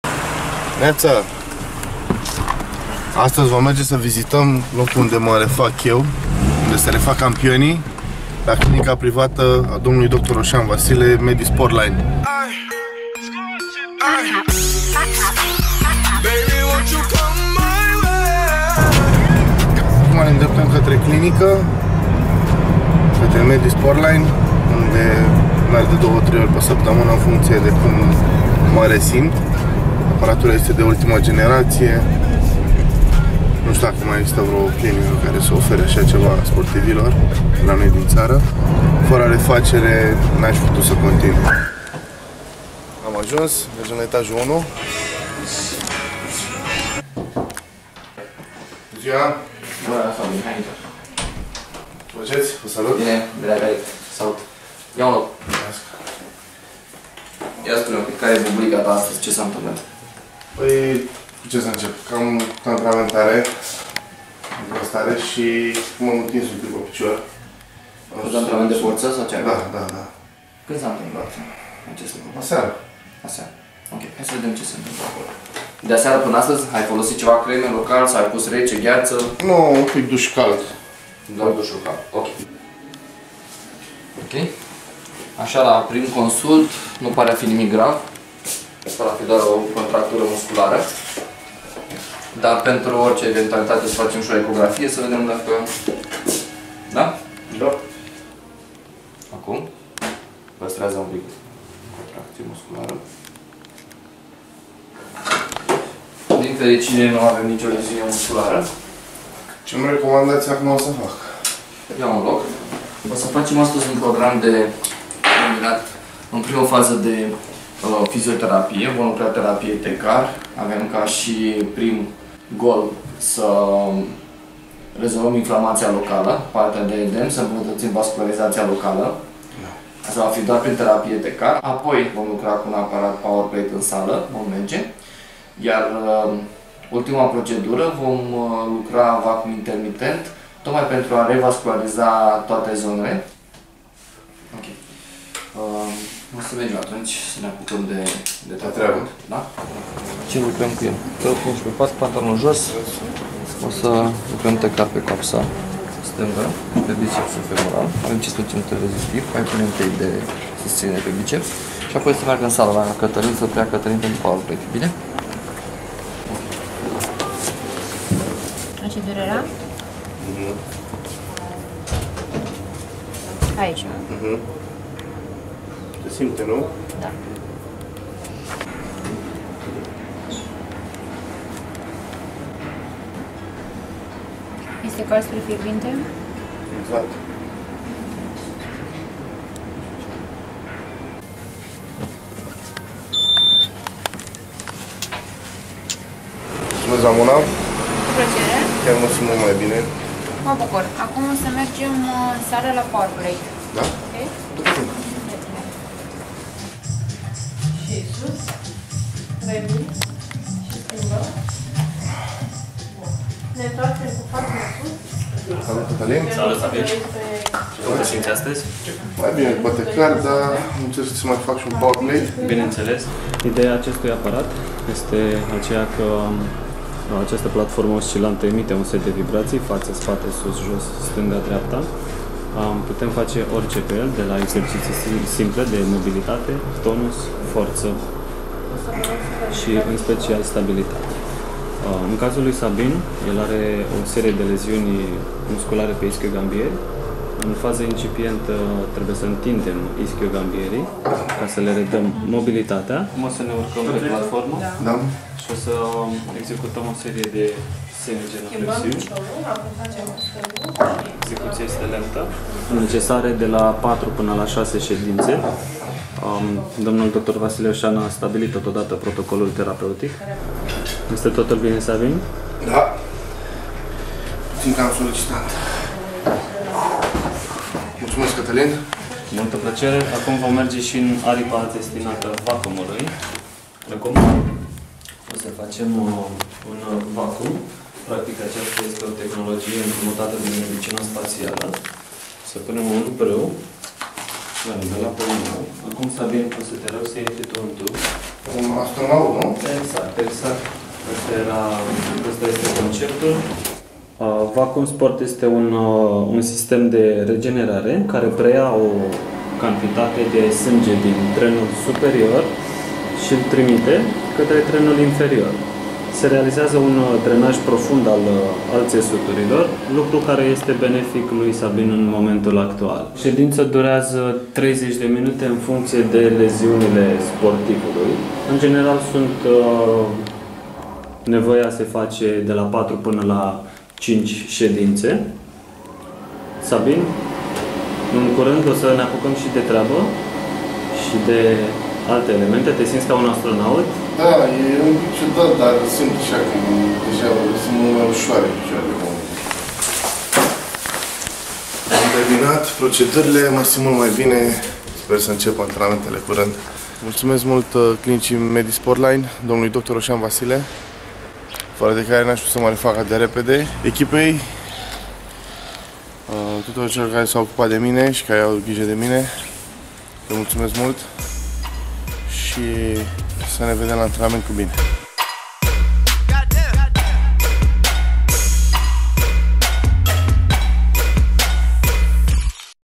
Refacere unui Campion - Episodul 3 - Medy Sportline (Dr. Vasile Osean)
In acest episod va prezint locul unde ma refac eu , la clinica privata a doctorului Osean Vasile , Medy Sportline .